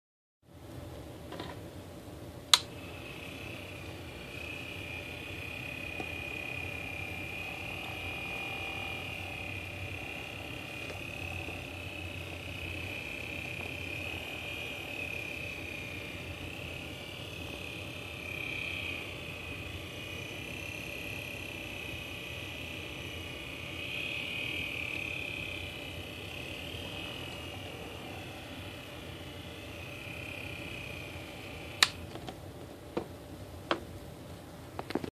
Custom Ghostbusters Giga Meter light FX demo
Demonstration of the light effects for my custom Giga Meter version 5.